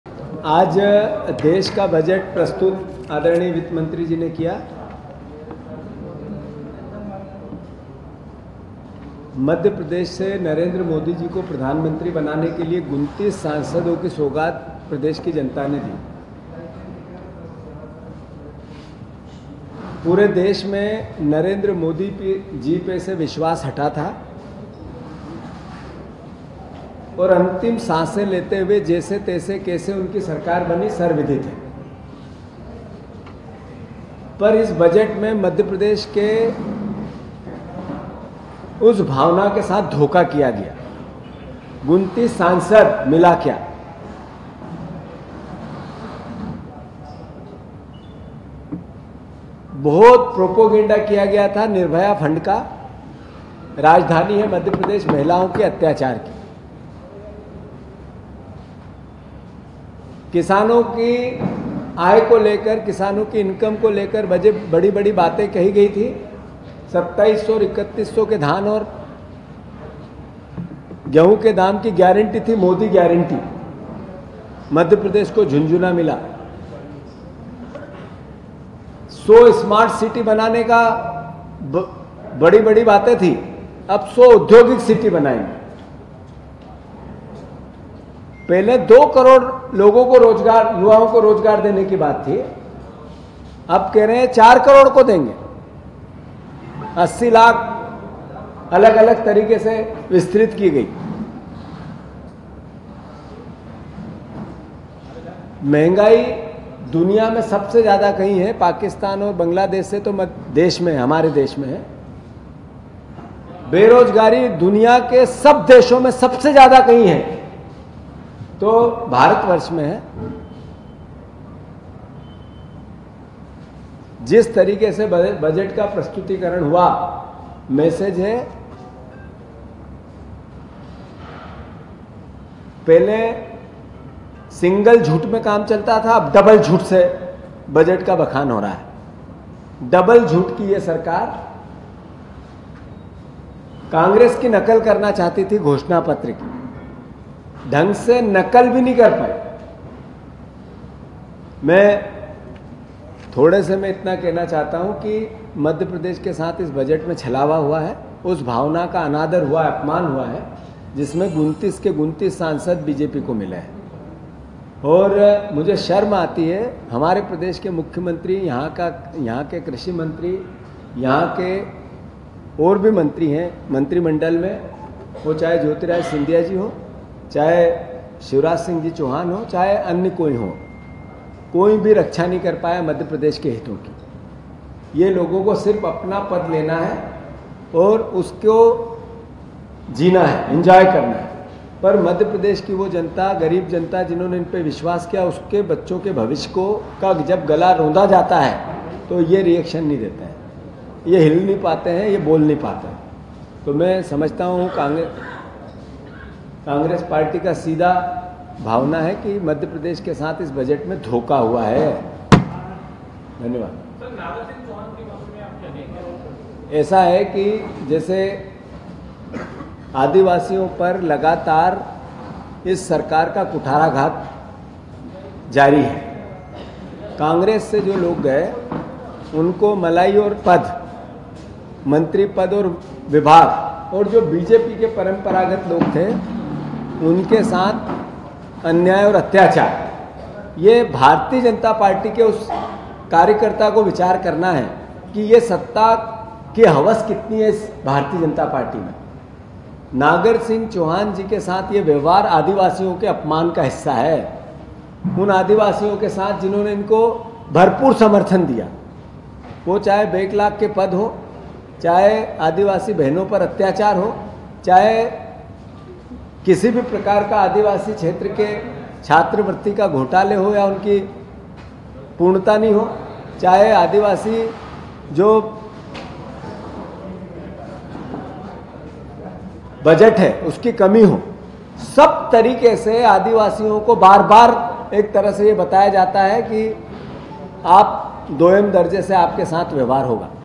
मोदी सरकार के बजट पर Jitu Patwari का रिएक्शन, बोले- Madhya Pradesh के साथ धोखा हुआ ।। Budget 2024
कांग्रेस नेता जीतू पटवारी ने मोदी सरकार के बजट को 'झूठ का बजट' बताया है. उन्होंने कहा कि सरकार 2 करोड़ रोजगार के वादे के साथ सत्ता में आई थी, अब वह 4 करोड़ रोजगार की बात कर रही है.

Congress leader Jitu Patwari has called the budget of Modi government a 'budget of lies'. He said that the government had come to power with the promise of 2 crore jobs, now it is talking about 4 crore jobs.